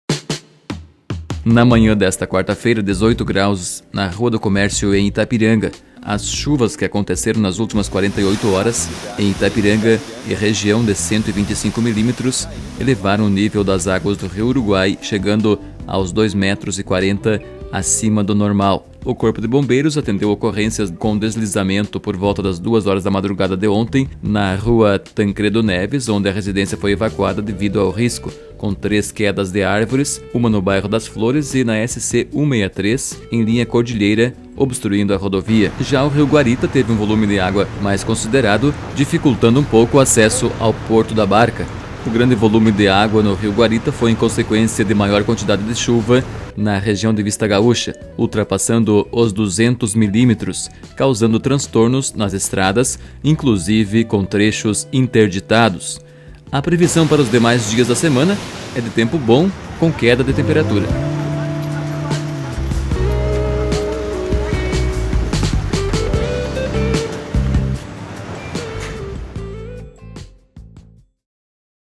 Nível das águas no Rio Uruguai e Guarita com queda de temperatura
Chuvas chegam à 200mm